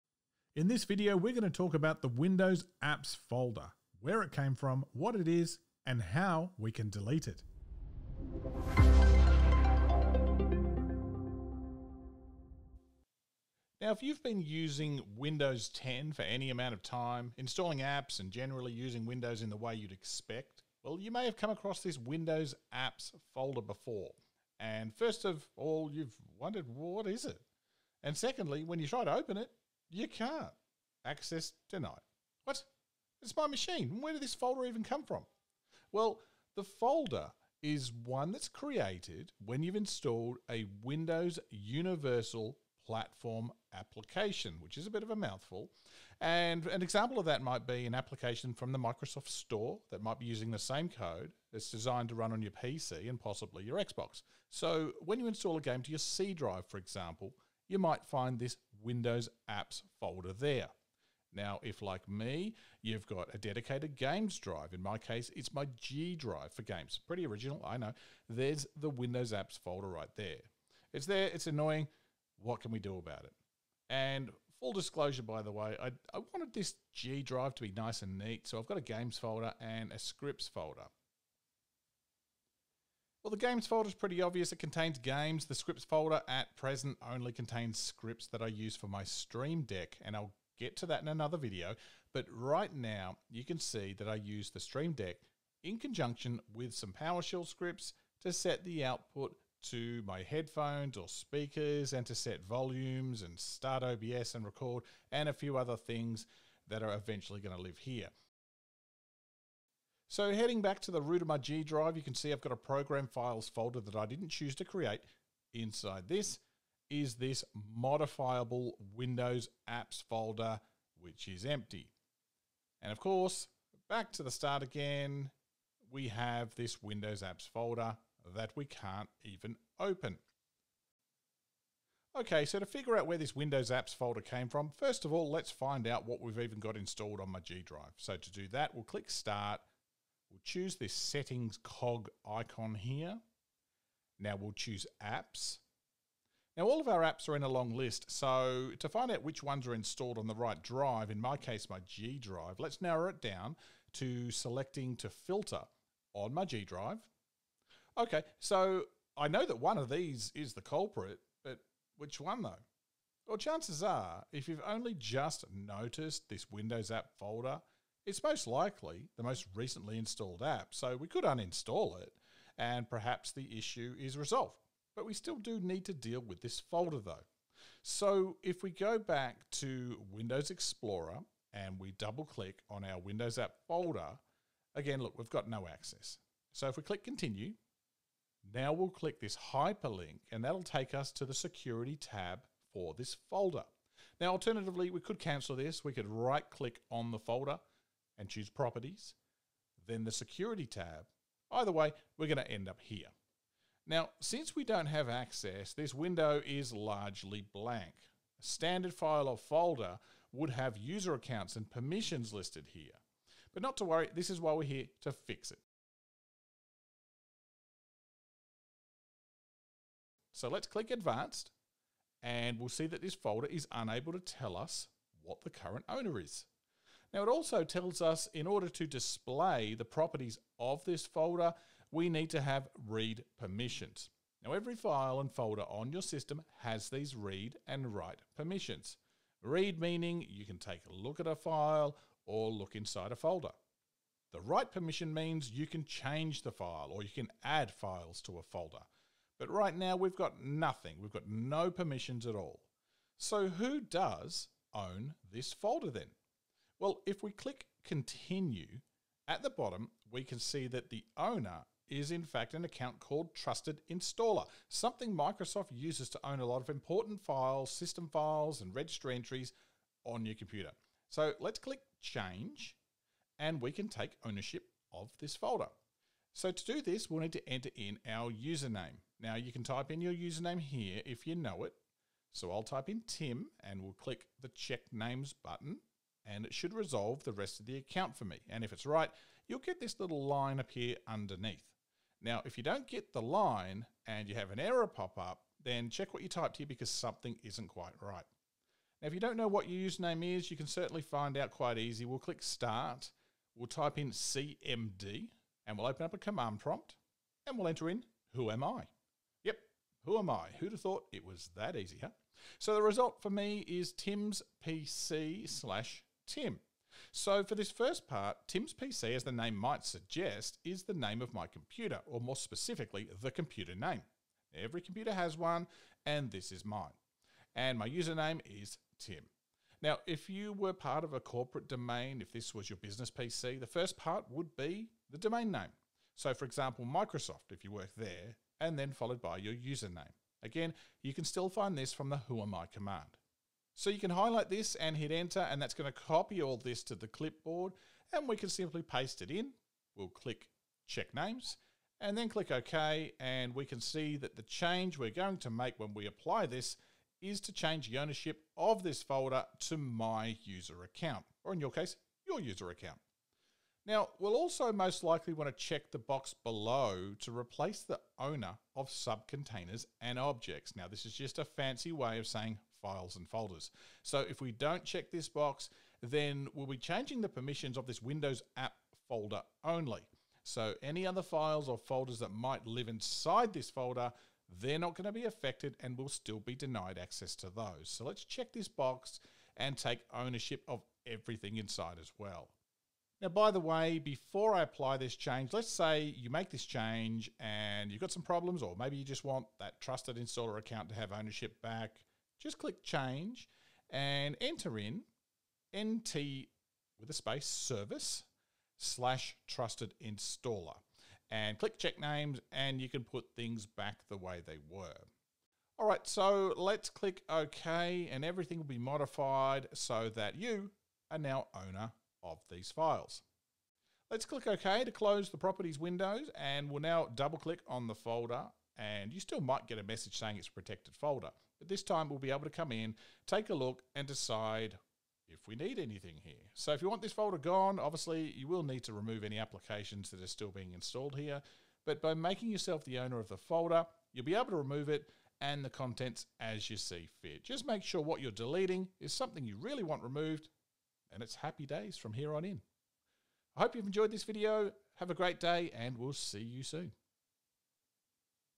How to delete the WindowsApp folder on Windows 10
Did this WindowsApp folder magically appear on your Windows 10 PC? Can't delete it? Let us show you how to take ownership of that folder (or any other file or folder) and delete it.